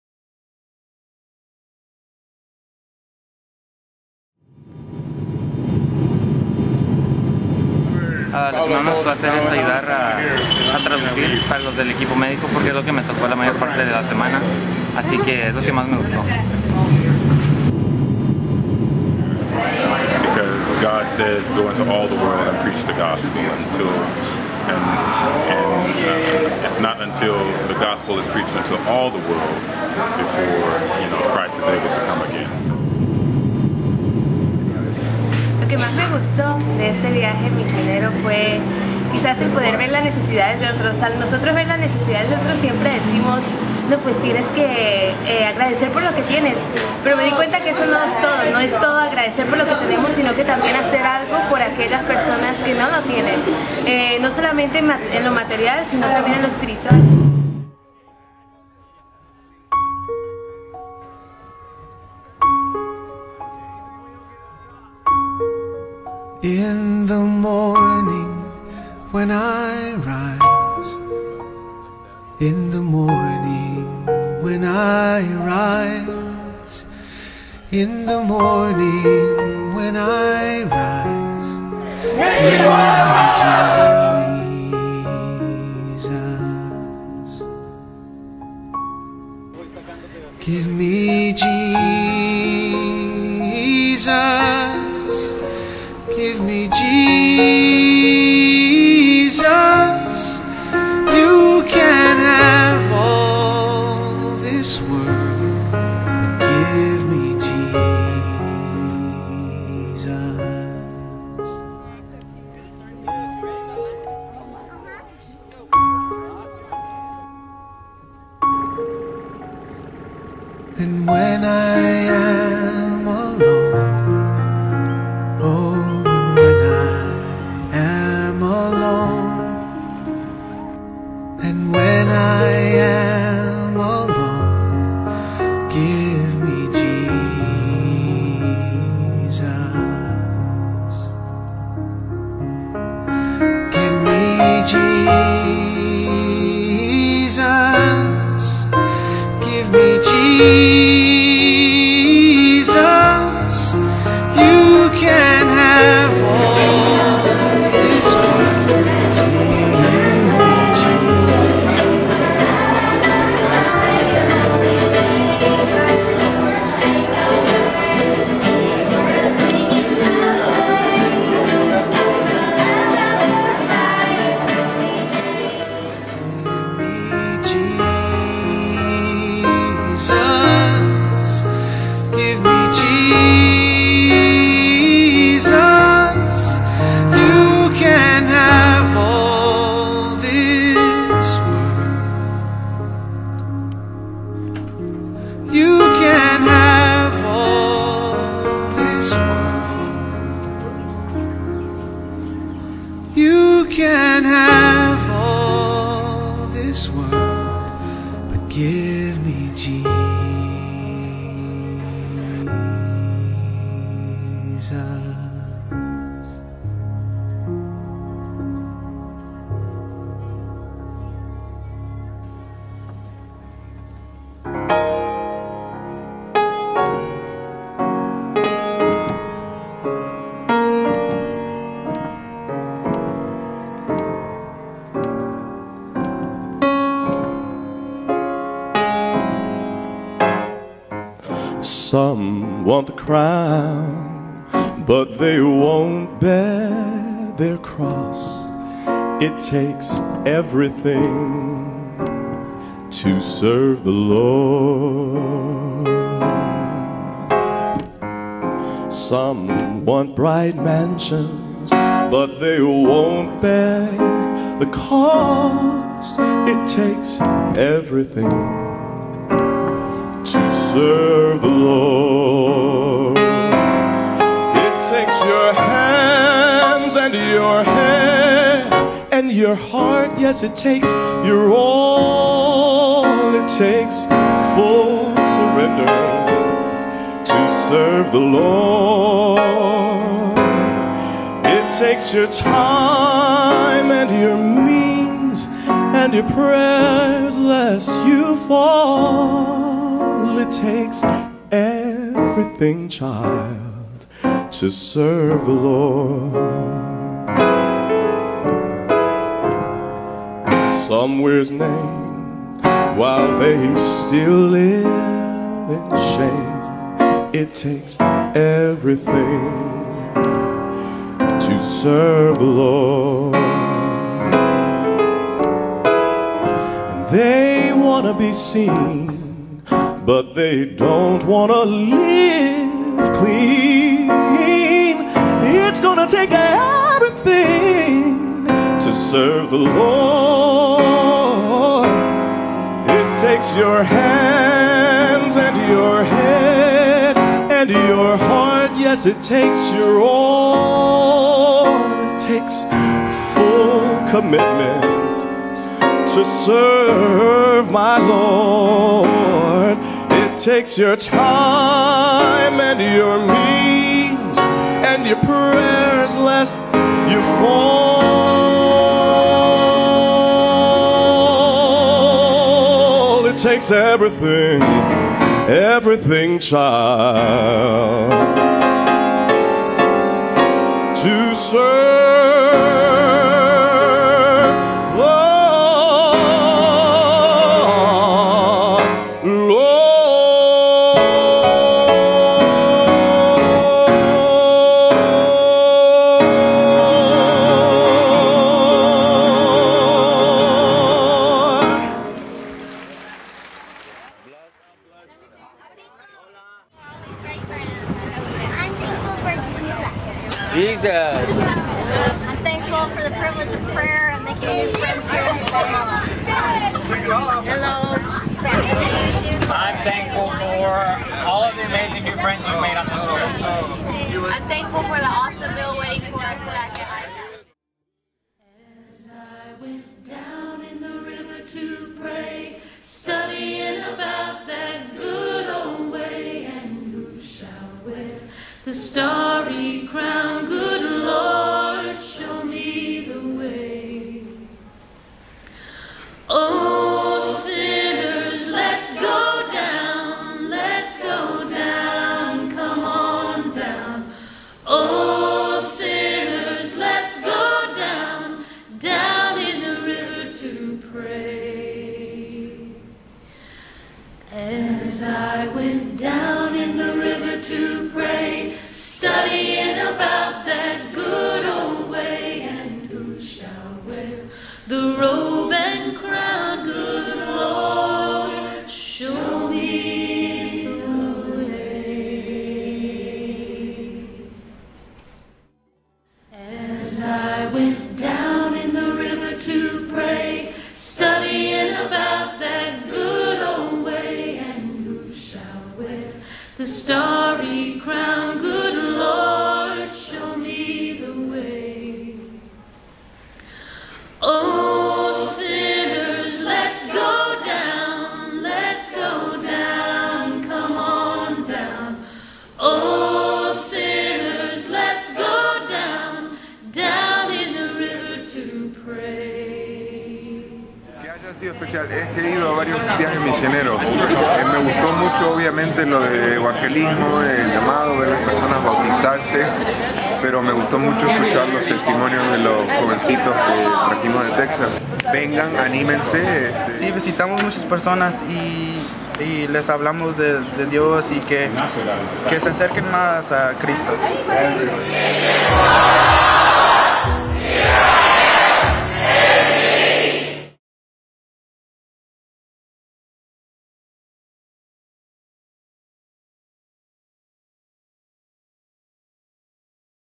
Mission 4 Guatemala 2015 HD captioned
A mission team visited Poptun, Guatemala for 10 days during the Thanksgiving holiday of 2015 with three purposes: to perform medical clinics in different areas of the village, to present evangelistic series on three sites, and to assist with the construction of a building for a soon starting nursing program at the ICAP.